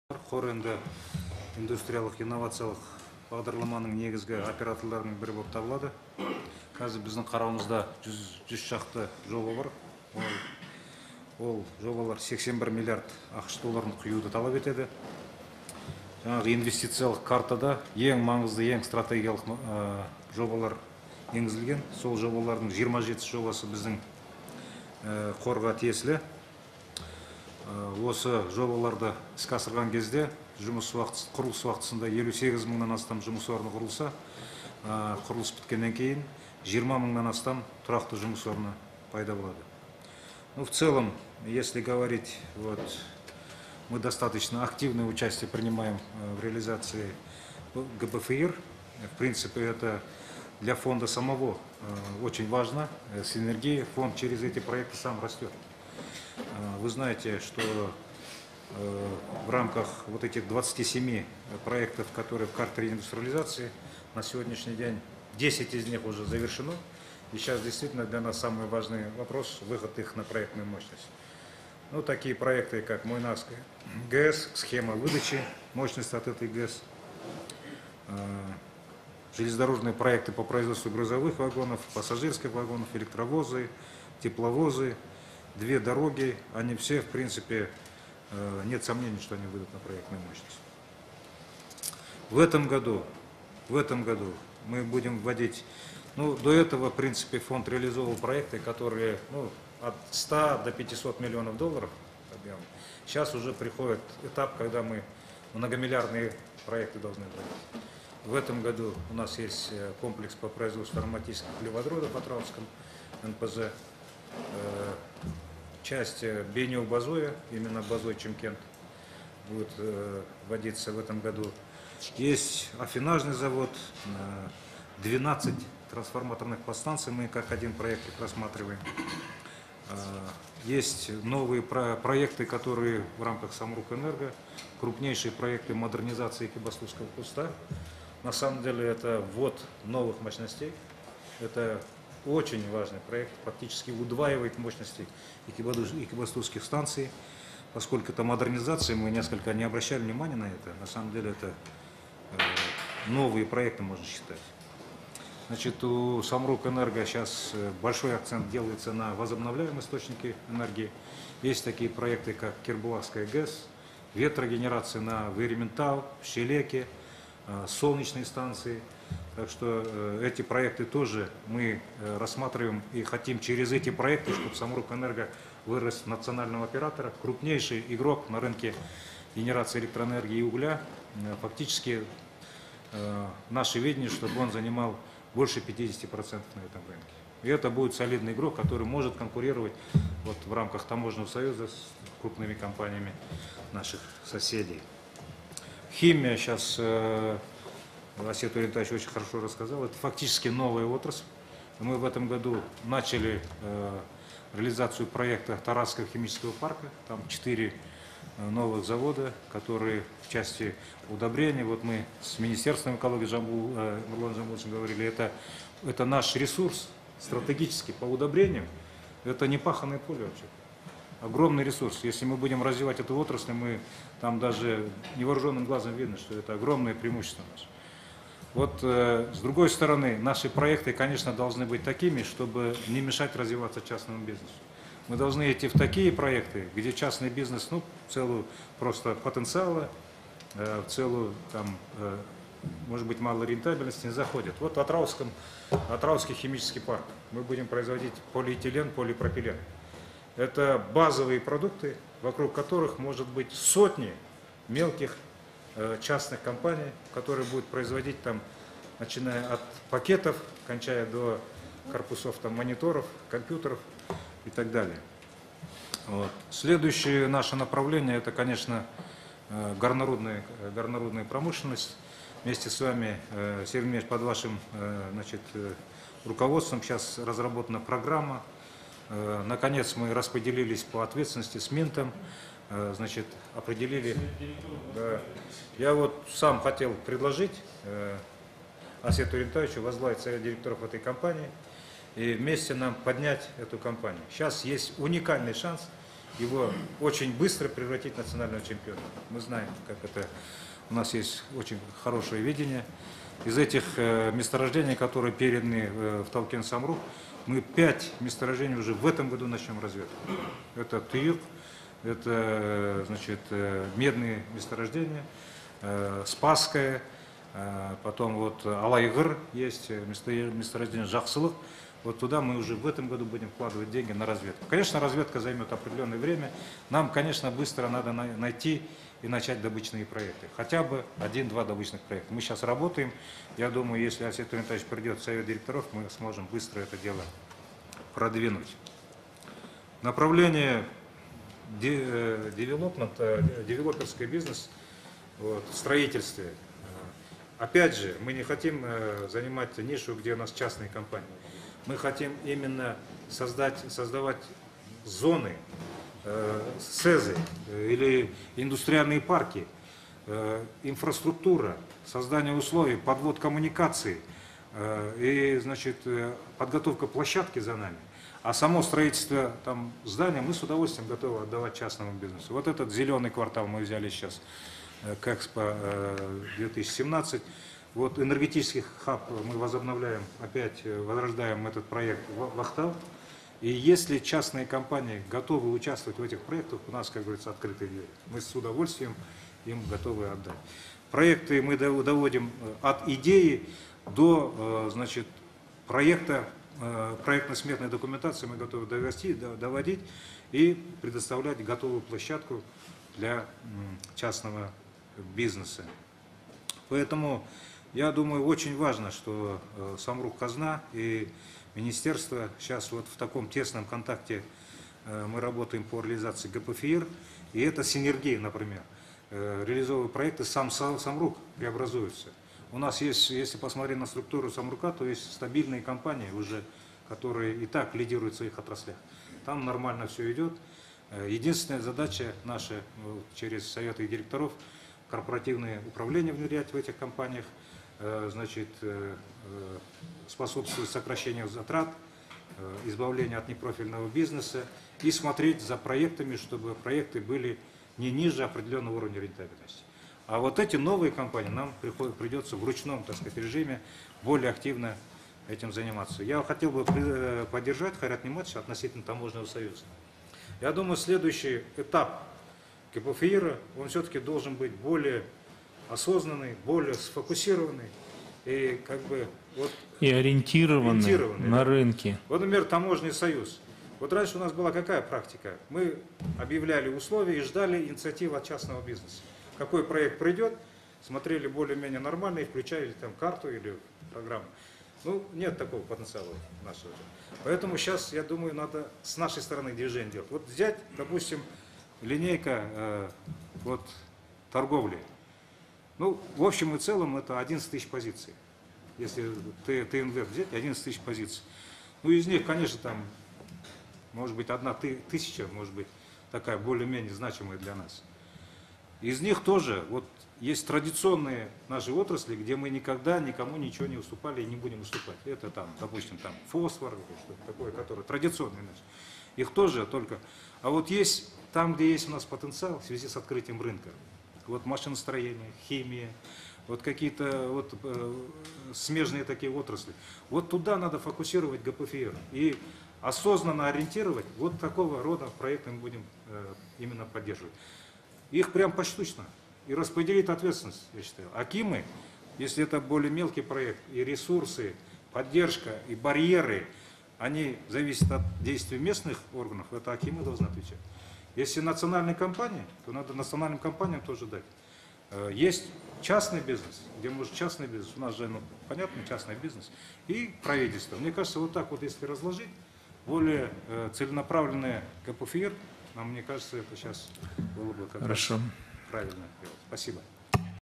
Выступление У.Шукеева
На заседании Правительства председатель правления «ФНБ «Самрук-Казына» У.Шукеев рассказал о деятельности предприятий Фонда в рамках ГПФИИР.